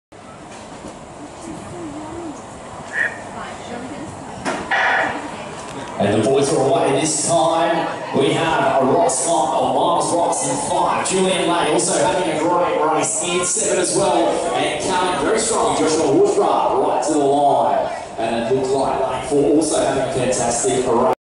Ht2. 100m Men, Alana Boyd Shield, QSAC 13/01/2018
1st. Jack Payne 10.80 w +1.2,  2nd. Julian Lay 11.00,  Stephen Karger 11.13.